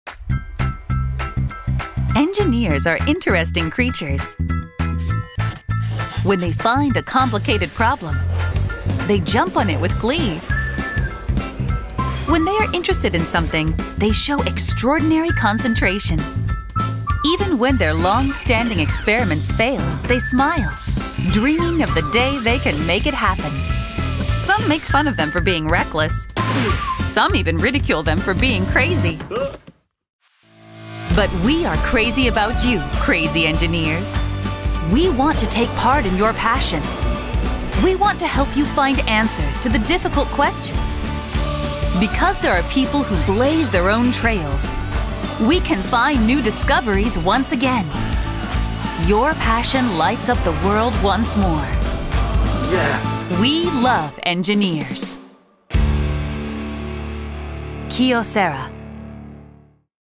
Kyocera Global Brand Movie: We Love Engineers (Full Version w/ subtitles)
Kyocera’s new brand movie for the global market (subtitle version).
Engineers are interesting. Some might not understand their passion. But because there are people who are willing to go off the beaten track, we can find new discoveries once again.
We Love Engineers.